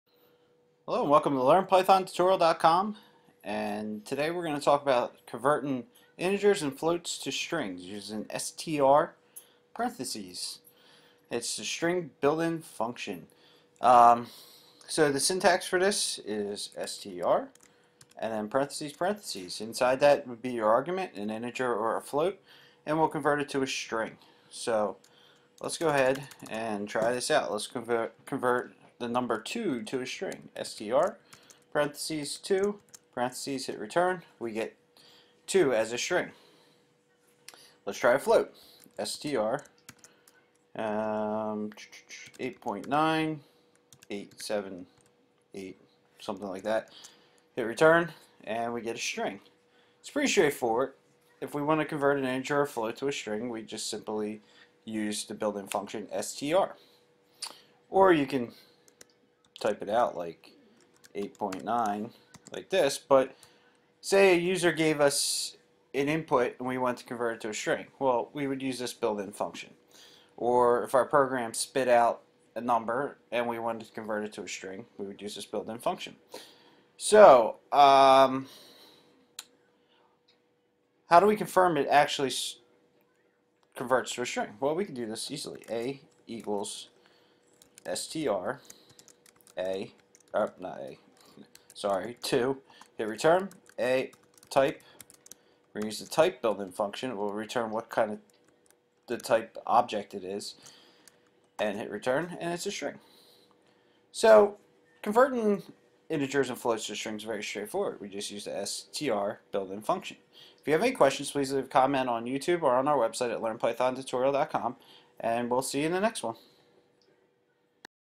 str() built-in function in Python
In this Python tutorial we take a look at str() built-in in Python.  The str() built-in function will convert other types like integer, float, list, tuple and so on to strings.